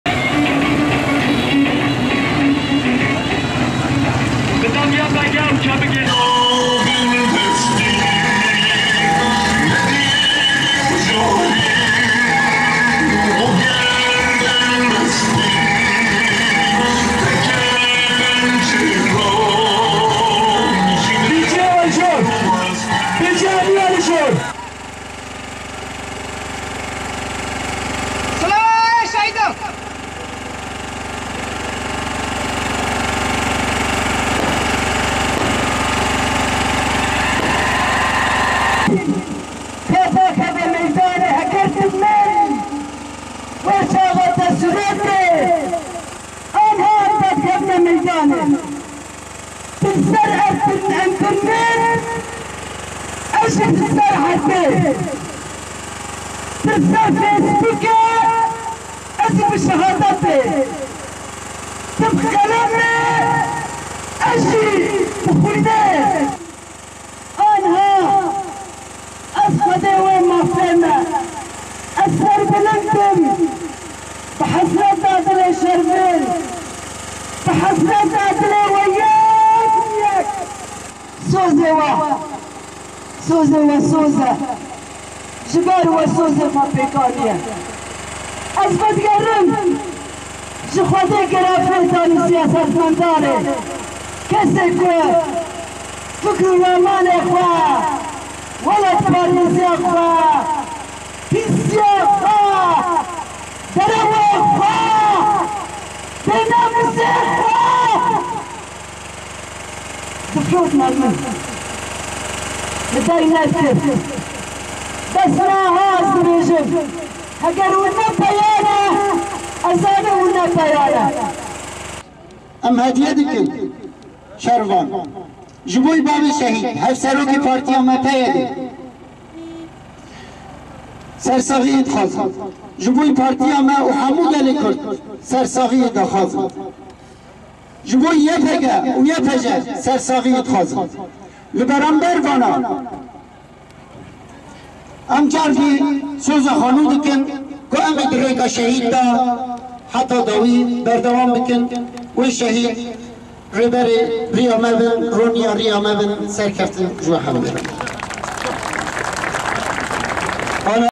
Onbinlerce kişi Şervan Müslim'i son yolculuğuna uğurladı
Türkiye destekli El Kaide çetelerinin 9 Ekim 2013 tarihinde  Tel Ebyad'daki saldırısında hayatını kaybeden PYD Eşbaşkanı Salih Müslim'in genç oğlu Şervan Müslim, bugün binlerce kişinin katıldığı bir törenle Kobani'de toprağa verildi.

Des dizaines de milliers de personnes se sont rassemblées le 10 octobre à Kobani pour les funérailles d'un combattant kurde. Il s'agit de Shervan Moslim, un fils du chef du principal parti kurde syrien PYD, Saleh Moslim, qui a été tué le 9 octobre lors des affrontements entre les combattants kurdes et les jihadistes d'Al-Qaida dans la région de Tall Abyad, sur la frontière avec la Turquie. Il s'agit de Shervan Moslim.